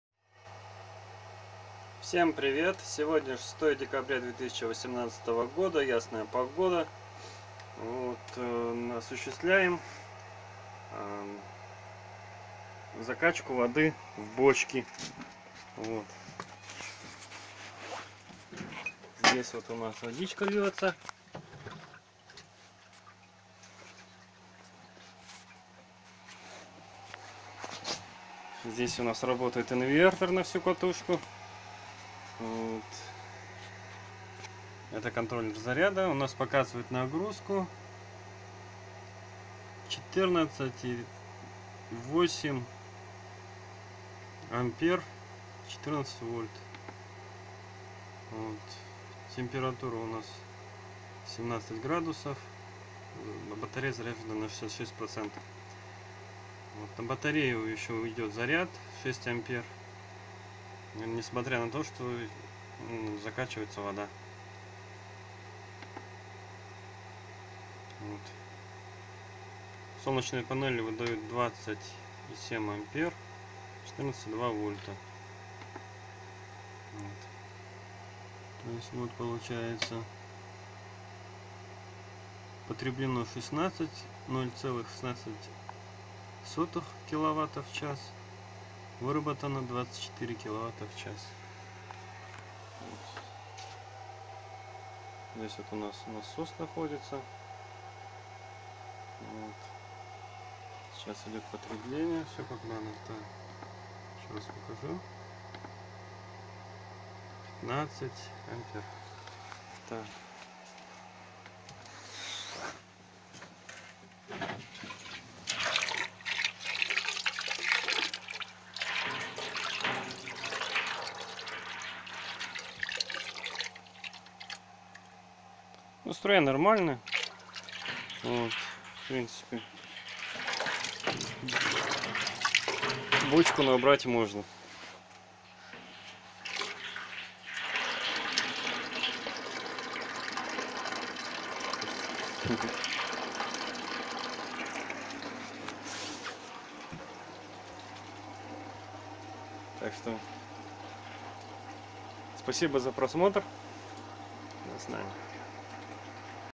Закачка воды зимой от Солнца
Закачиваем воду зимой в полдень исключительно  от Солнца с помощью электростанции на 500 Ватт и мембранного погружного насоса "Водолей". Энергии хватает и на закачку воды и на заряд аккумулятора.